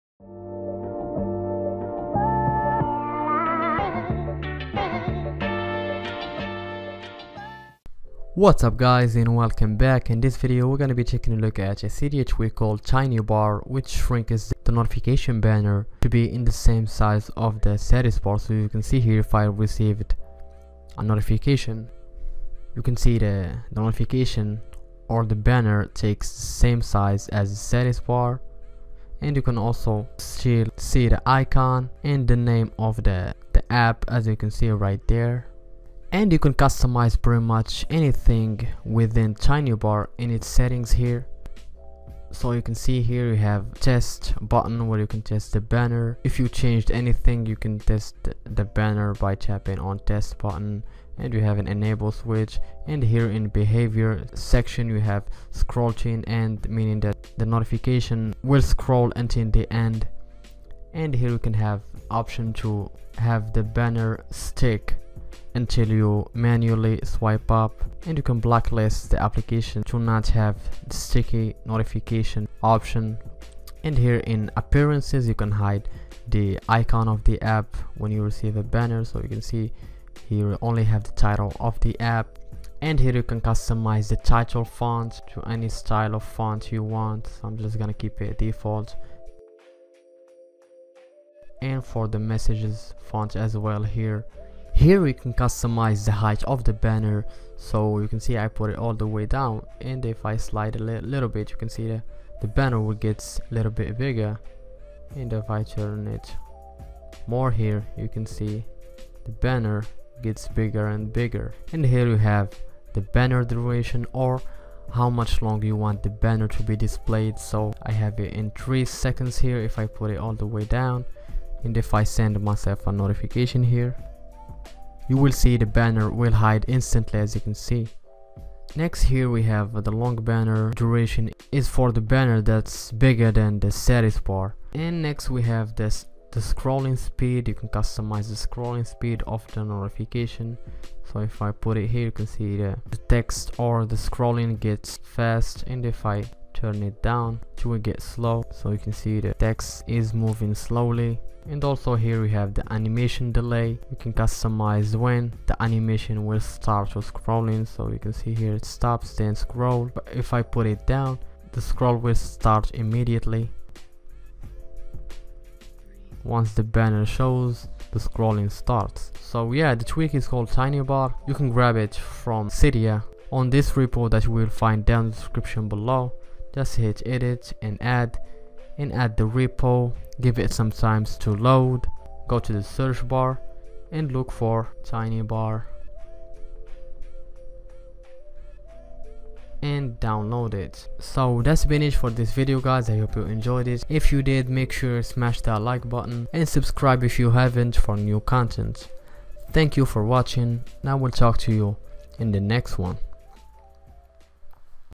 TinyBar: shrinks the NOTIFICATION BANNERS down to size!
Compatible with iOS 10 Jailbreak, TinyBar allows you to minimize the banner of the notifications to have a minimal look!